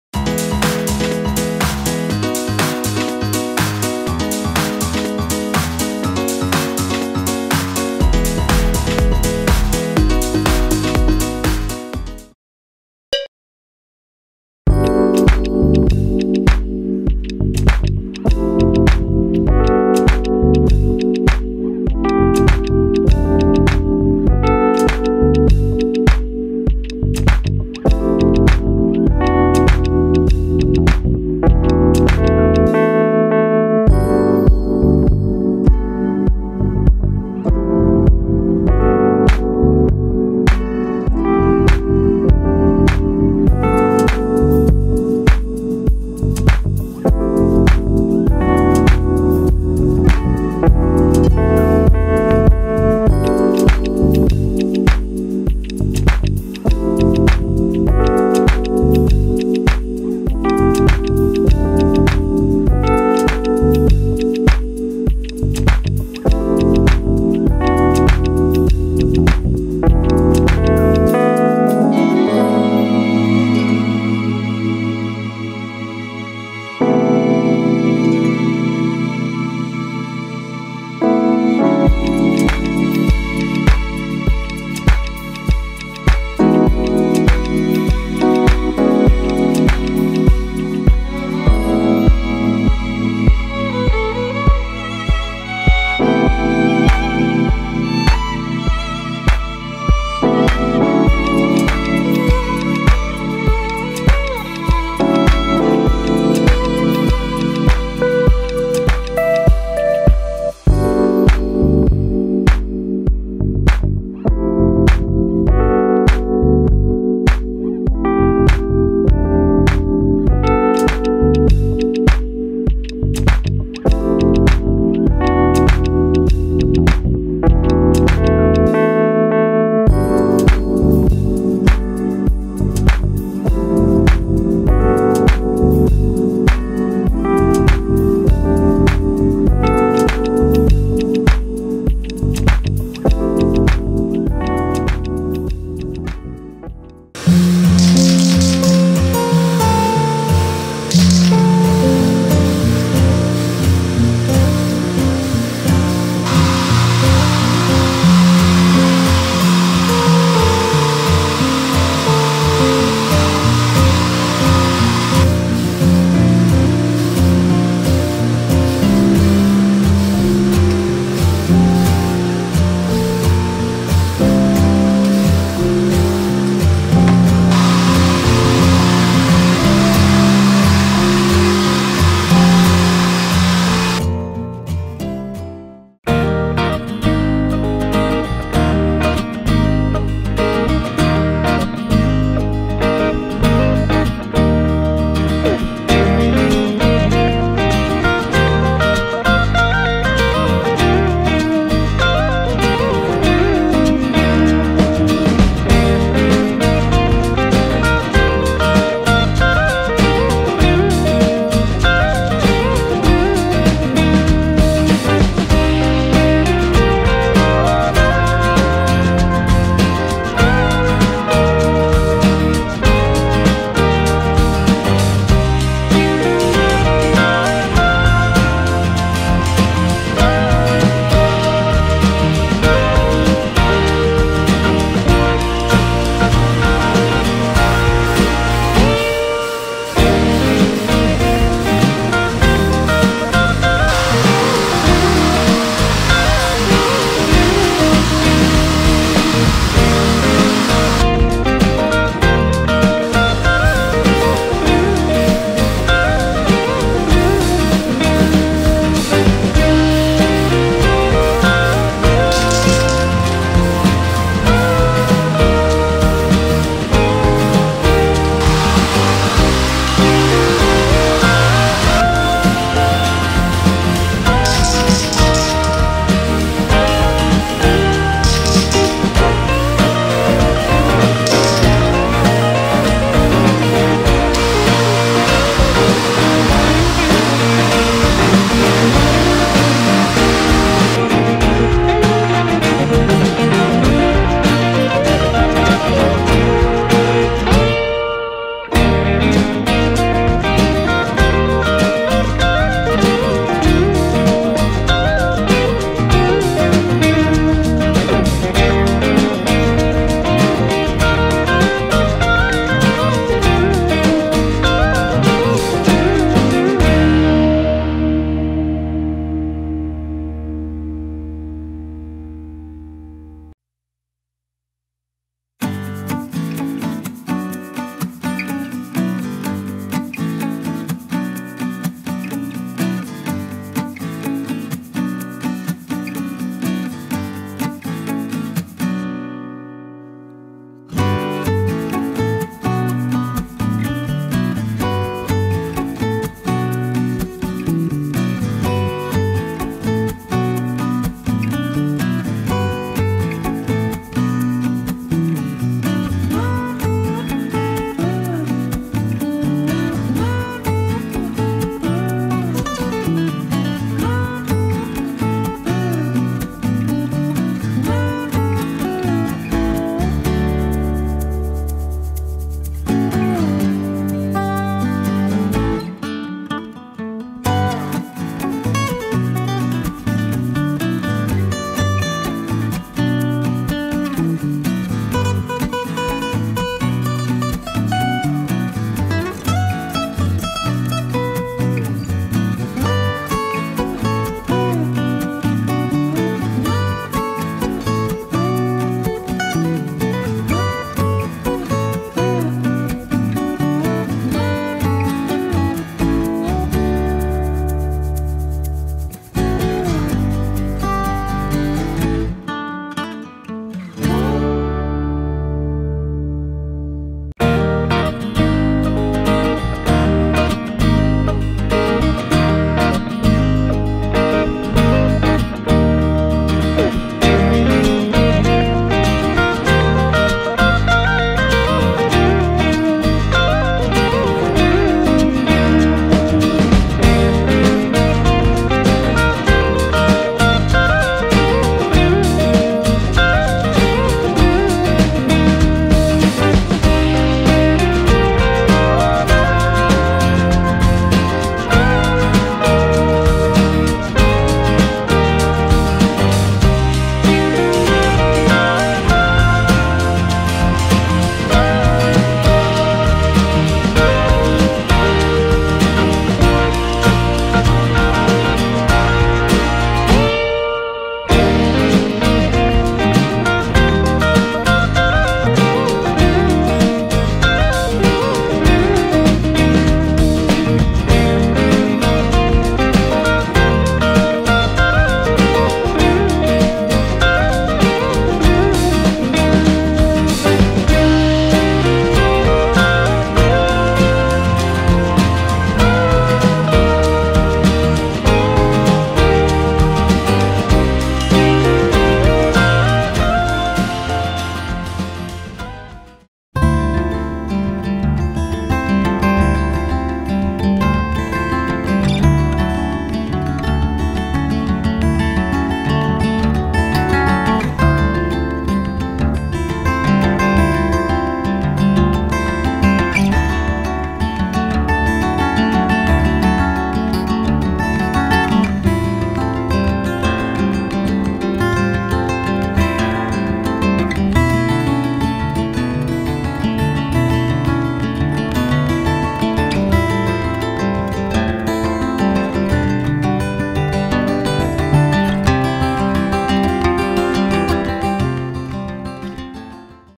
작업기 22_검빨 조합도 좋지만 검녹 조합도 아주 좋아요! 스펙터 2020 하이브리드 01 (타미야 미니카)
구독자 분의 댓글을 보고 그린 색상 위주의 색상 조합을 보여드립니다.
역시나 검녹 조합은 믿고 가는 무난한 조합이 아닐까 싶네요 ㅎㅎ
오랜만에 좋아하는 매드레이저 바디로 신차를 만들어 보아요.